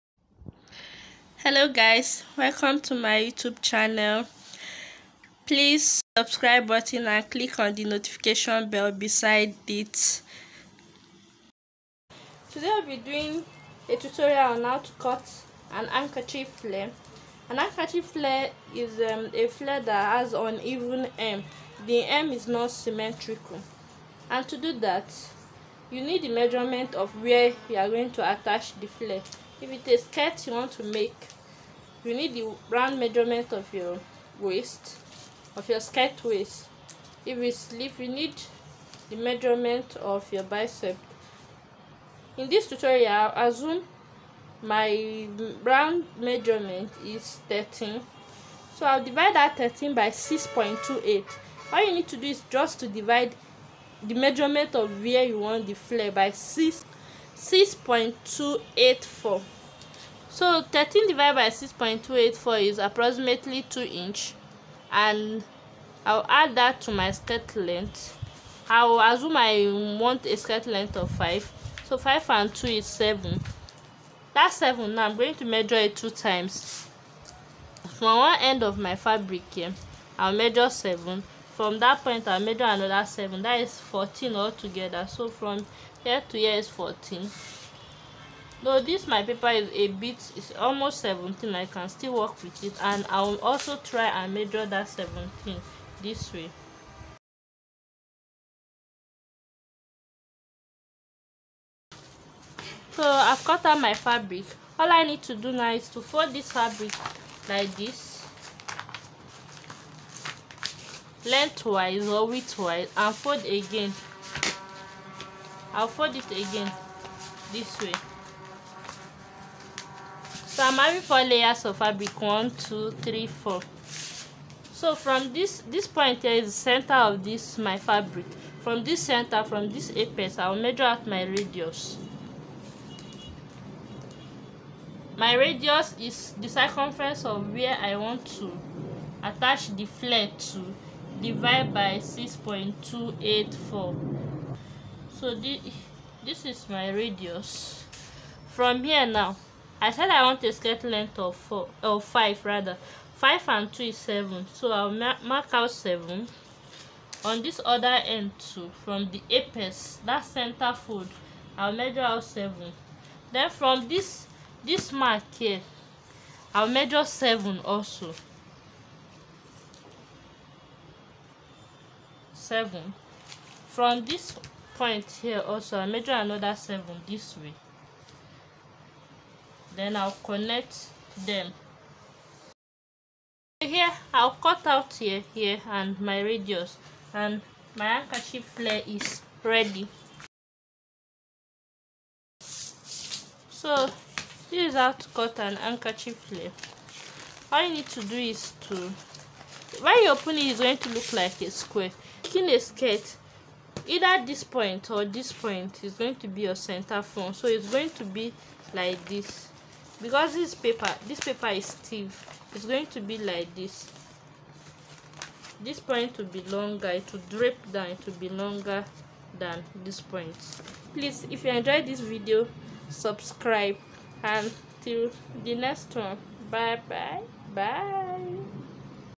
HOW TO CUT HANDKERCHIEF FLARE
This video teaches all about handkerchief flare which is a flare with uneven hem.